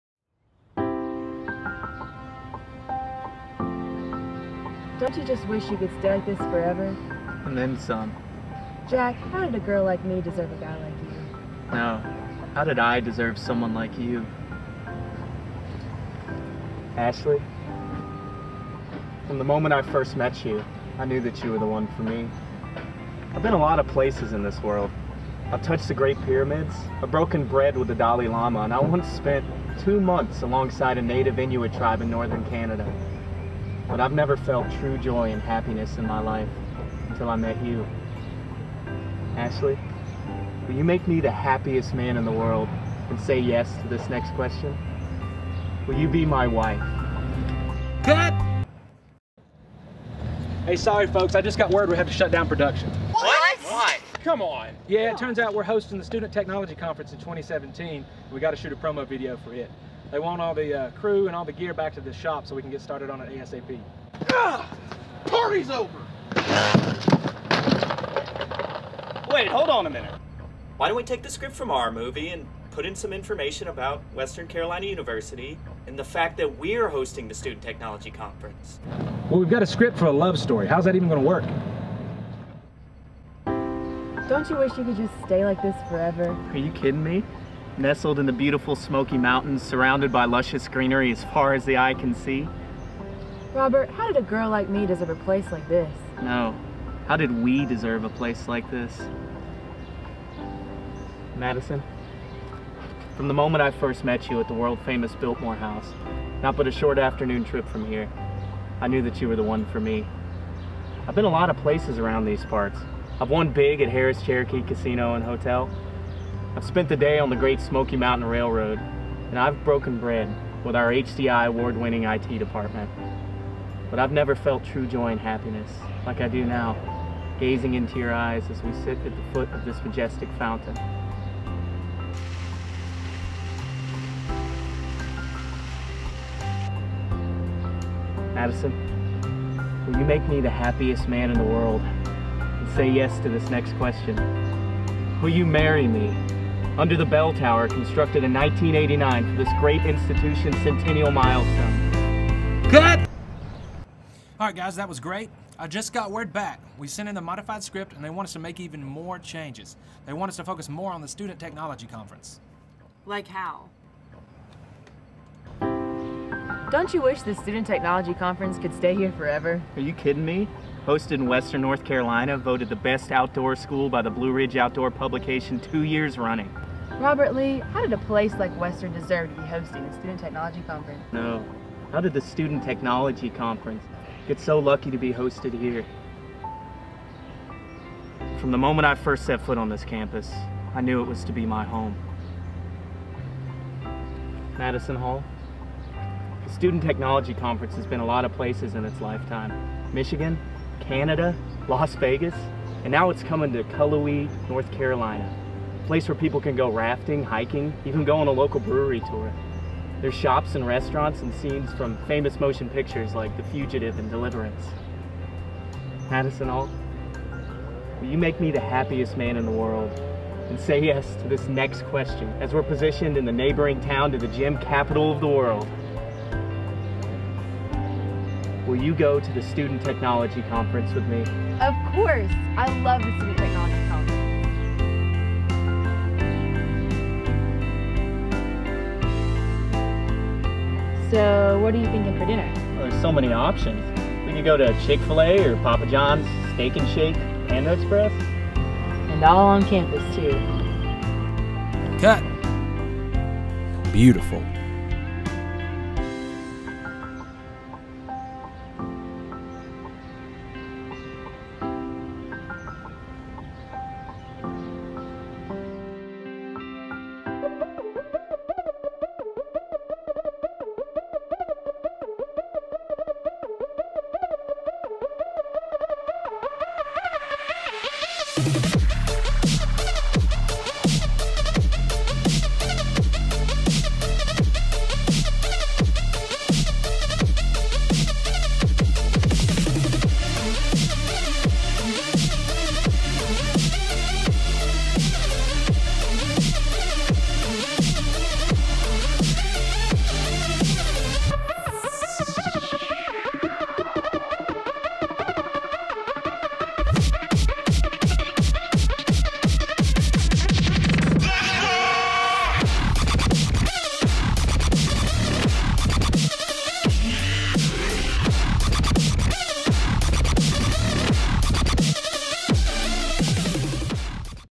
WCU Student Technology Conference 2016 Announcement Video
We are excited to announce that Western Carolina University will be hosting the Student Technology Conference in 2017! Learn more about the Student Technology Conference